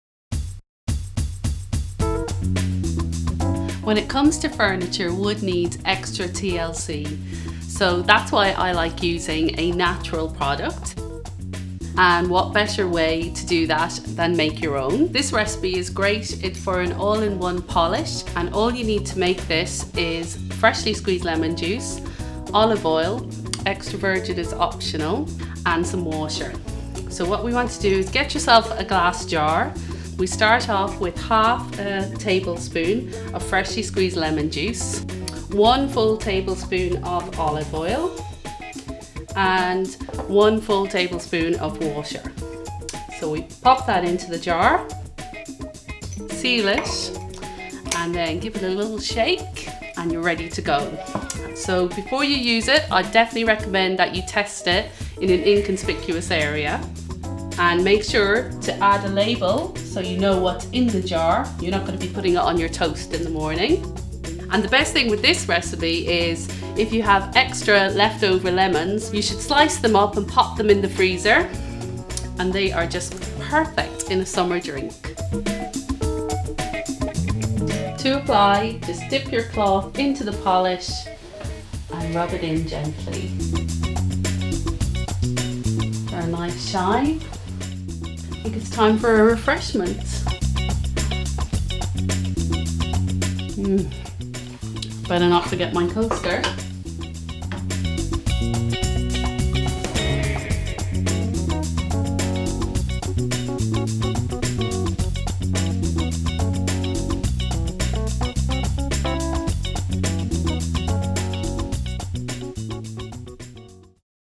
All in one Polish Green Cleaning Life Hack
Get cleaning the natural way with our chemical free, easy to make All in One Polish.

Find out more about the Roscommon County Council’s Green Cleaning Life Hacks